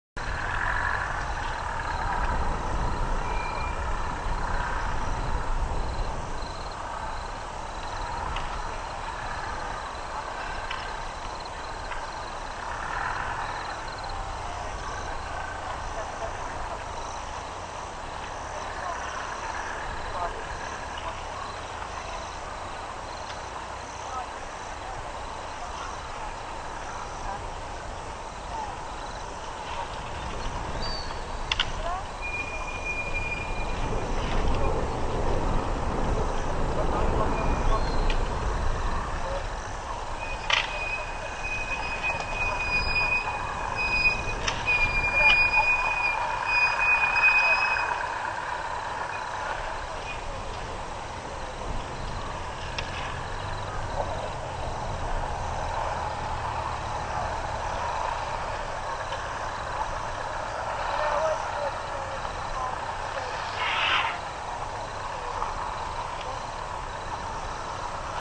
Fireside chat without chatters; Sep 5 2010 6:13-6:14PM SAST
It's pre-FSC...they show the three chairs somehow. 1min -- video